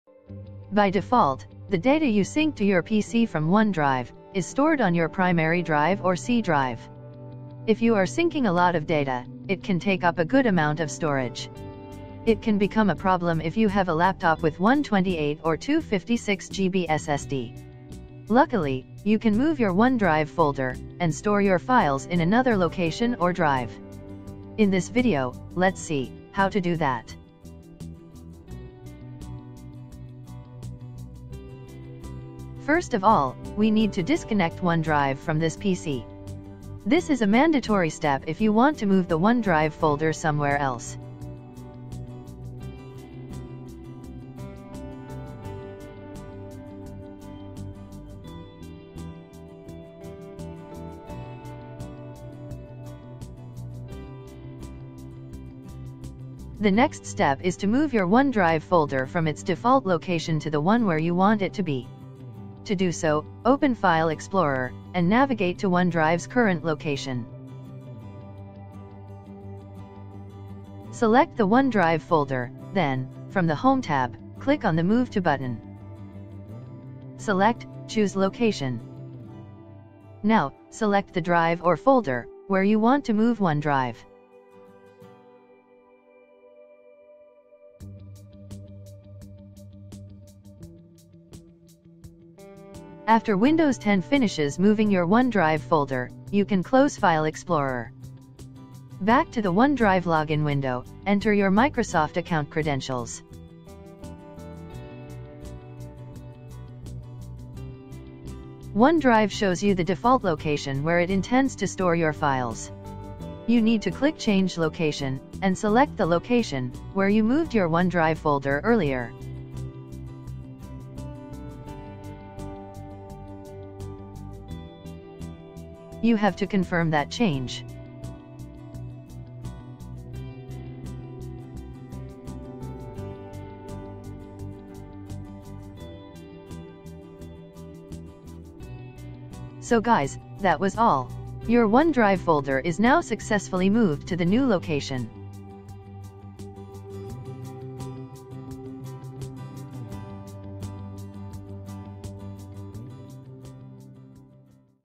Change OneDrive Folder Location on Windows 10
By default, your OneDrive folder is found in your user folder in Windows 10, but you can change where it is located. However, it isn’t a simple cut-and-paste operation; you have to make sure the new location syncs to the cloud. Here’s how to move your OneDrive folder to a new location on Windows 10. 💡More tech tips on our second channel!